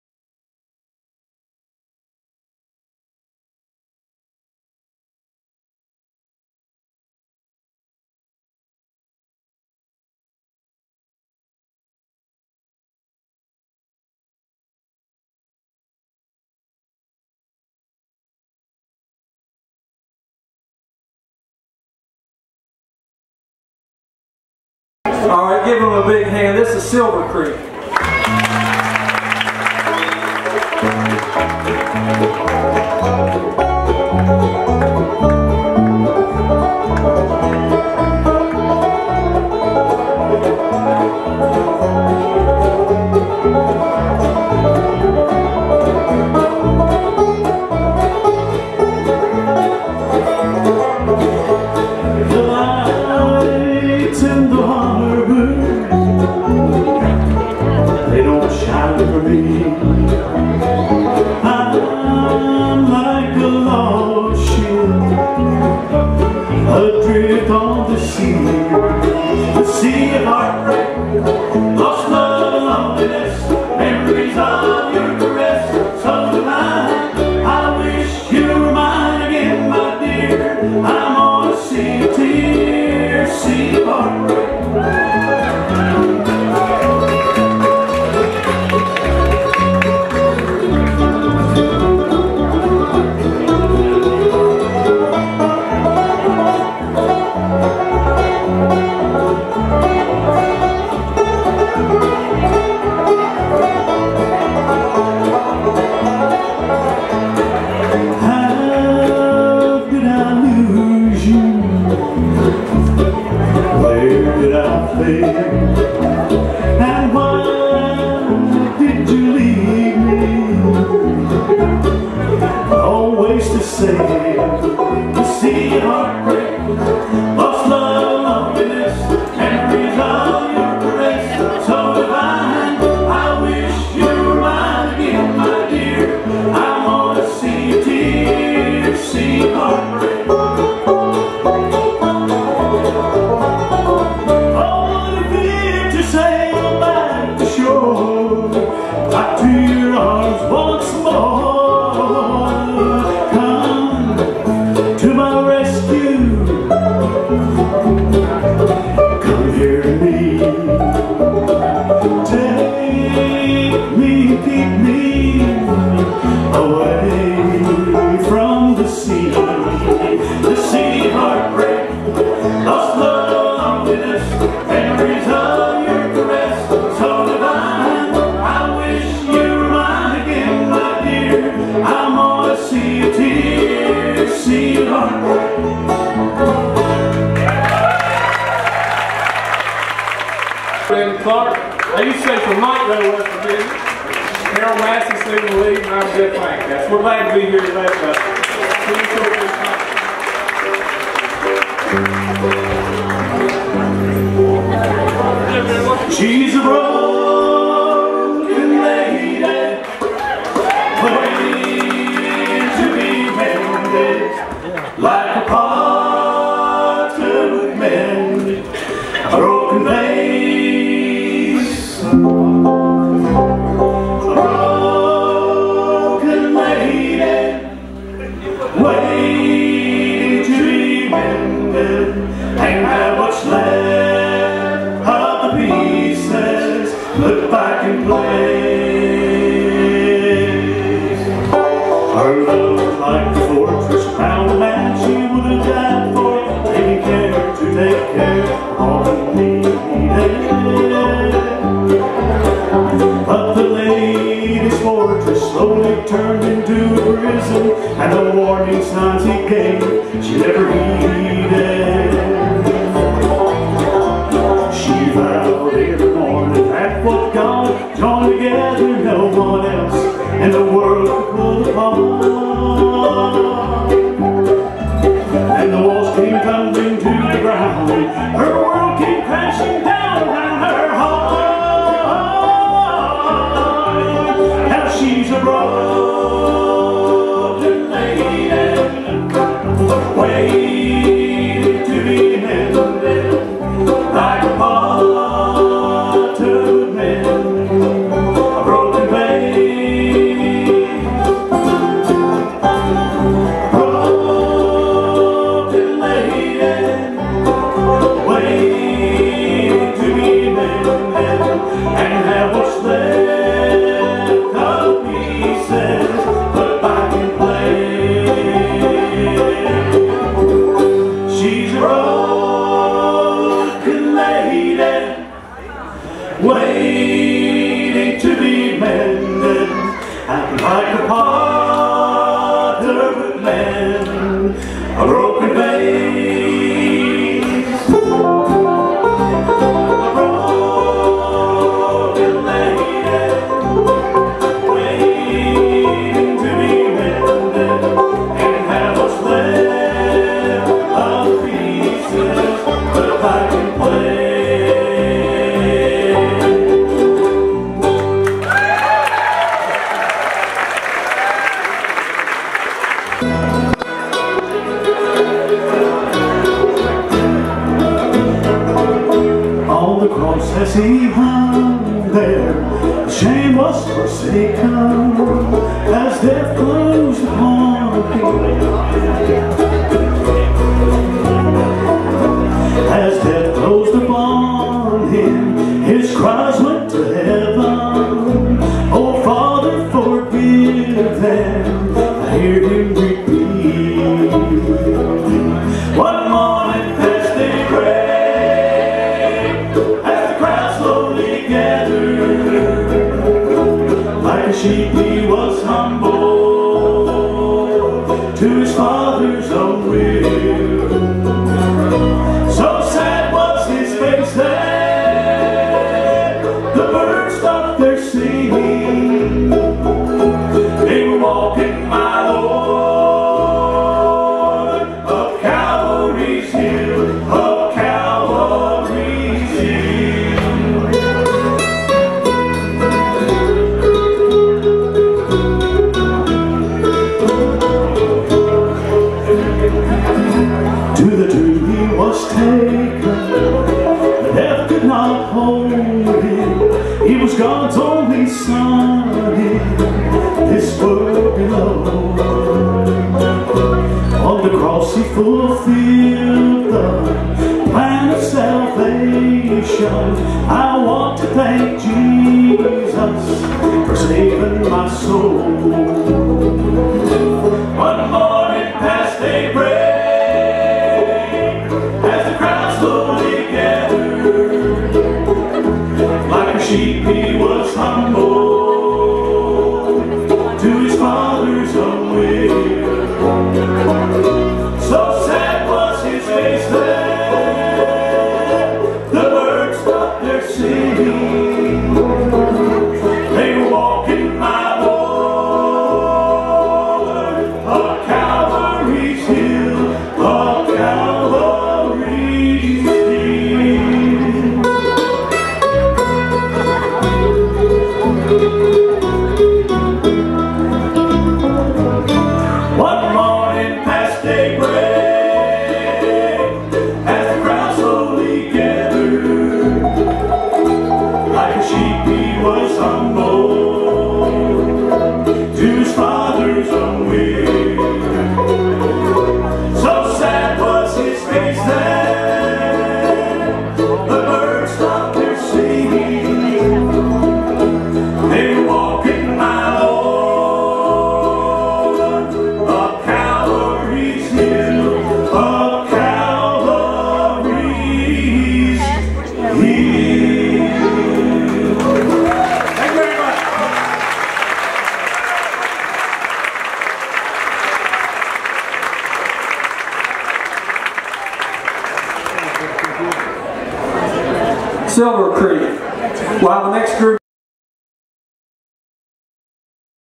Silver Creek Reunion
On June 2, 2018 - Randy Clark and original members from his band Silver Creek got back together and performed at the Mountaineer Opry House in Milton, WV.
They were part of the last show for this iconic venue after 46 years of performances and service to the community.